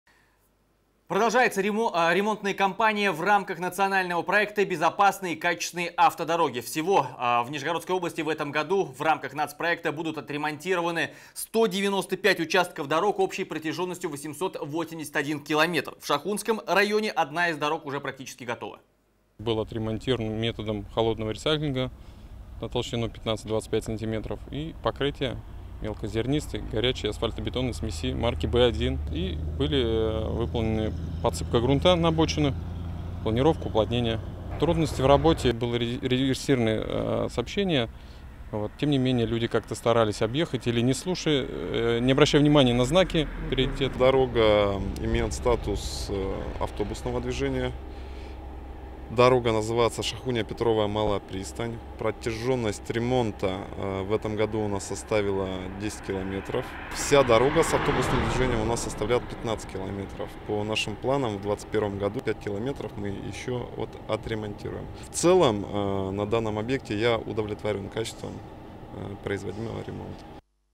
В Шахунском районе отремонтирована дорога в рамках национального проекта
Продолжается ремонтная компания в рамках национального проекта «Безопасные и качественные автодороги». Всего в Нижегородской области в этом году в рамках национального проекта будут отремонтированы 195 участков дорог общей протяженностью 881 км. В Шахунском районе одна из дорог уже практически готова.